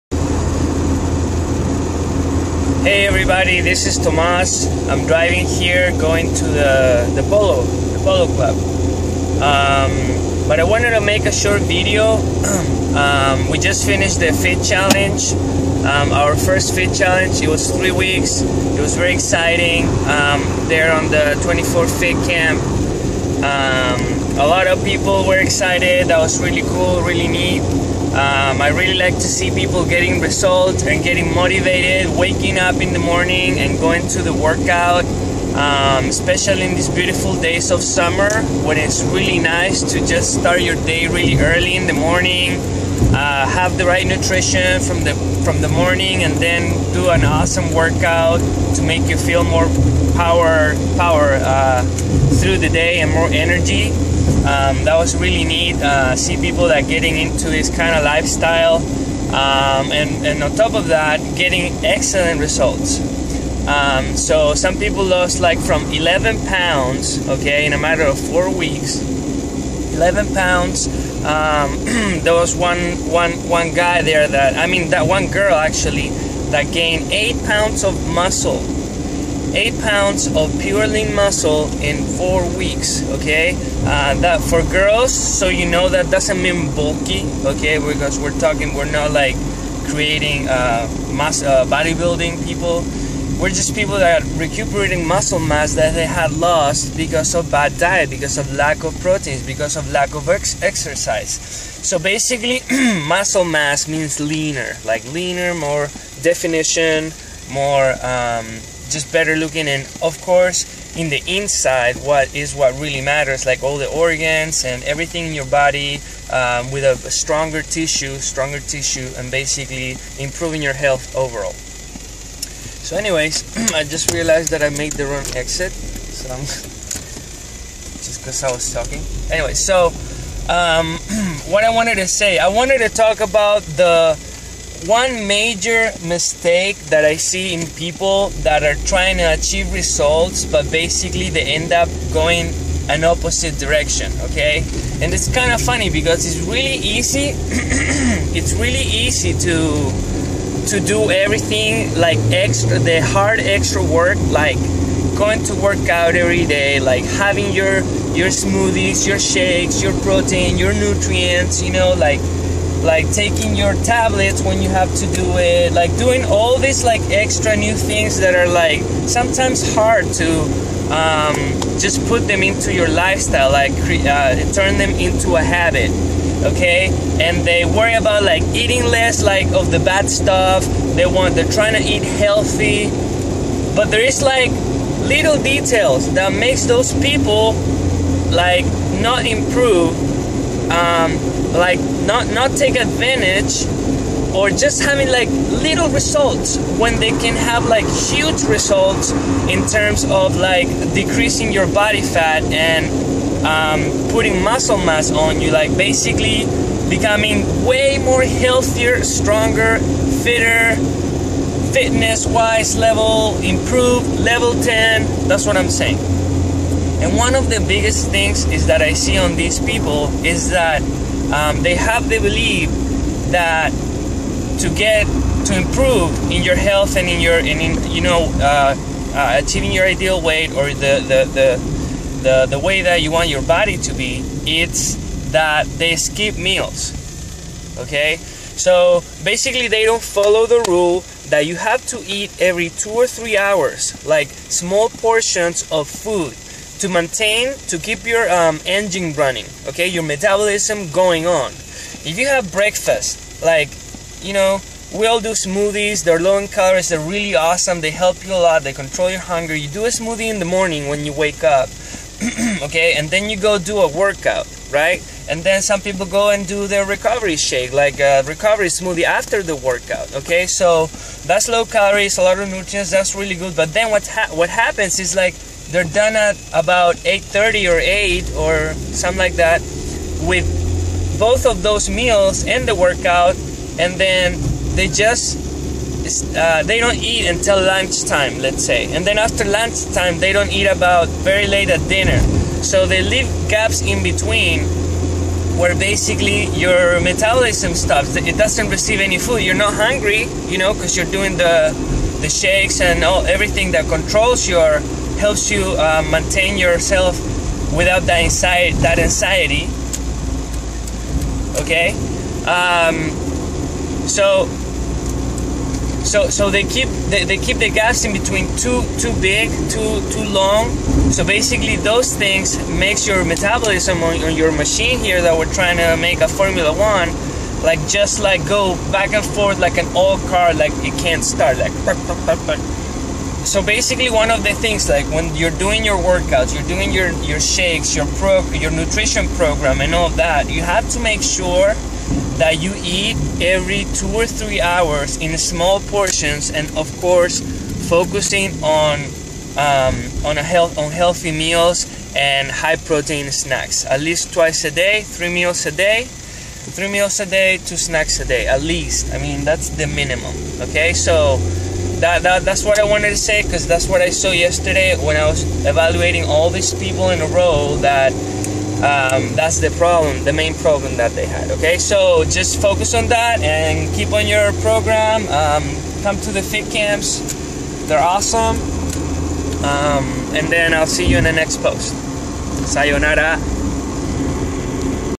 Biggest mistake for not loosing weight.
In this video I talk about the biggest mistake that people make when they're trying to loose weight by working out and having a nutrition plan This is Tomas Murphy Wellness coach for the 24-fit.com